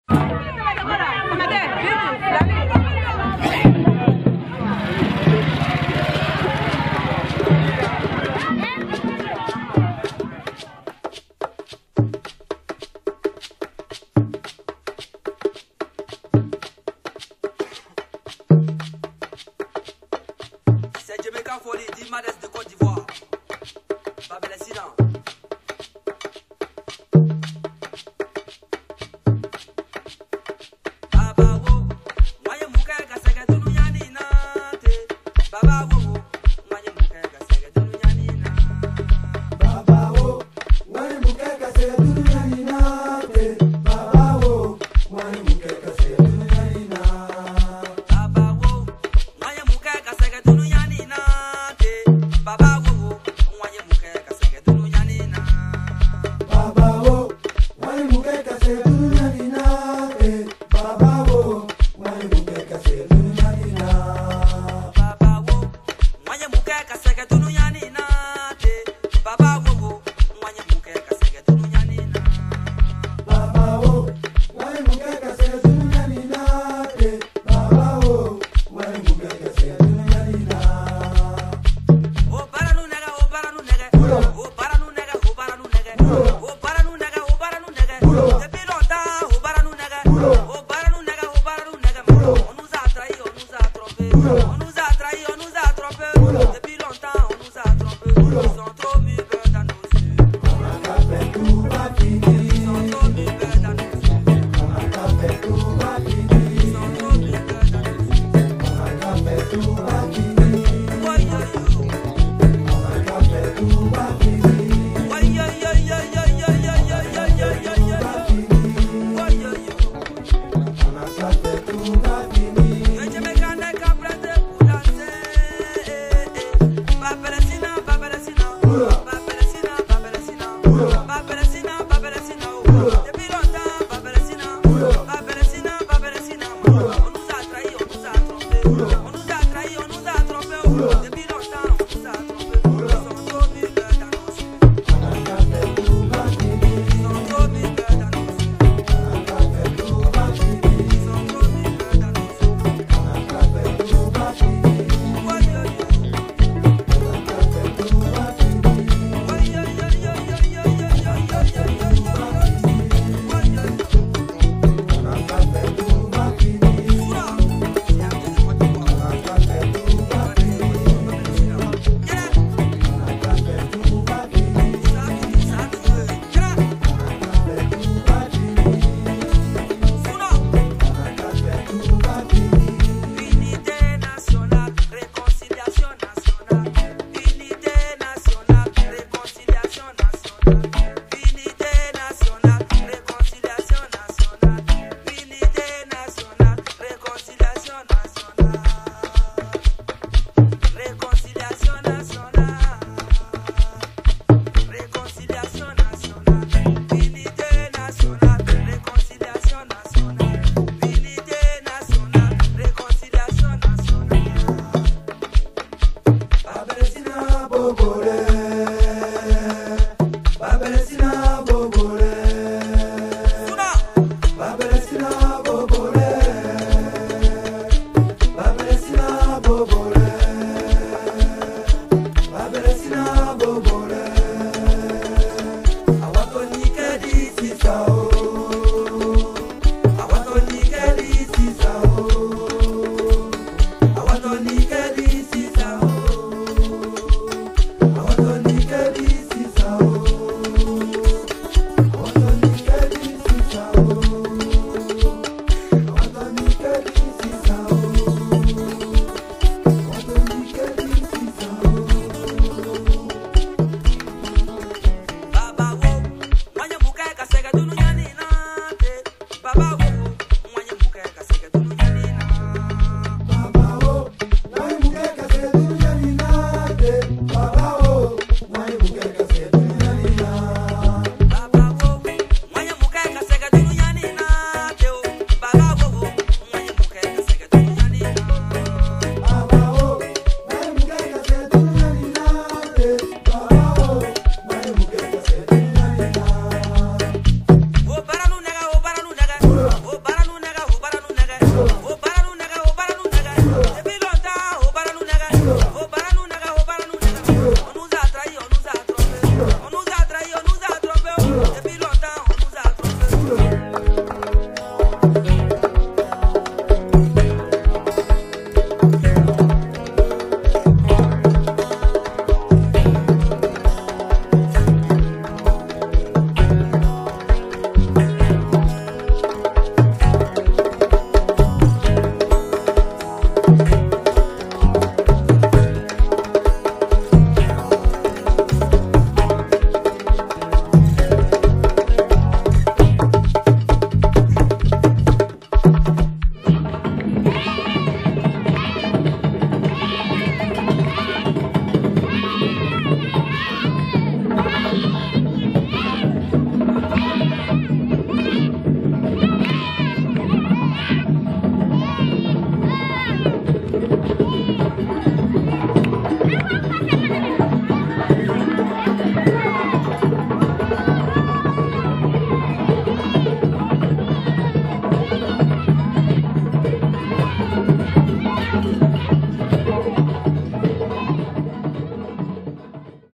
Les enfants de Kela
Filmé et enregistré en 2007 au MALI, dans la région de Koulikoro à Kela, village des Griots, à 80 km, au sud de la capitale BAMKO, ce vidéo clip est un hommage à la joie de vivre et à la gaieté de ces enfants.